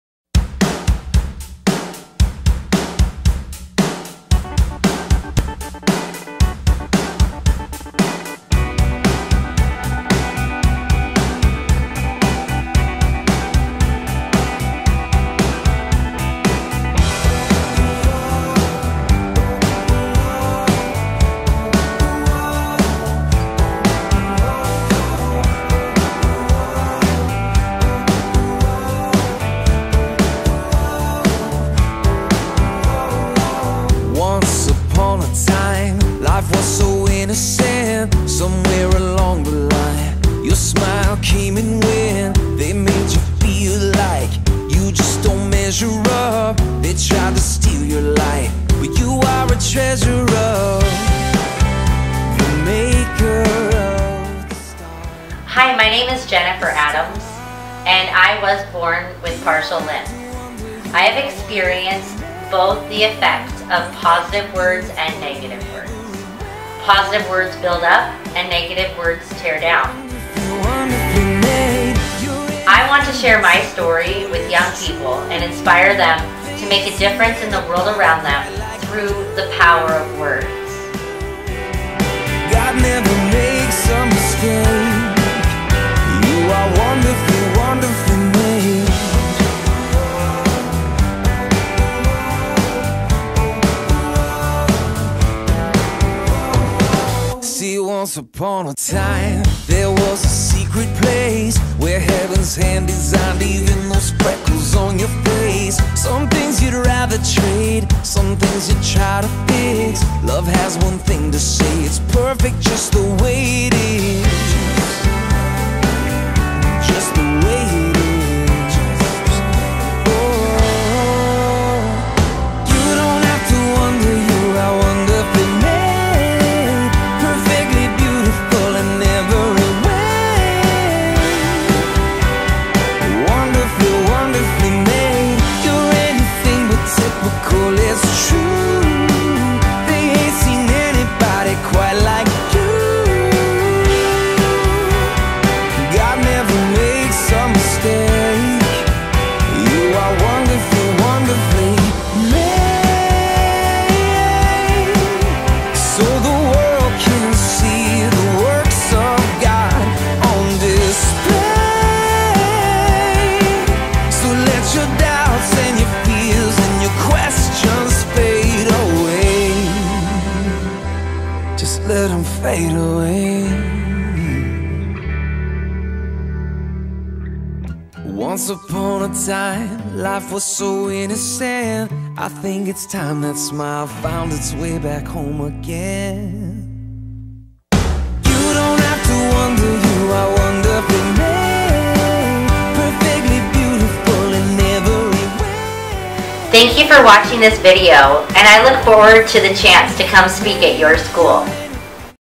Words Have Power Church Video
Jenny has a vision for a revolution in schools
which inspired her to start the anti-bullying campaign "The power of Words." Her story and dynamic presentation will draw in students and powerfully impact their lives. To learn more about her story and services visit her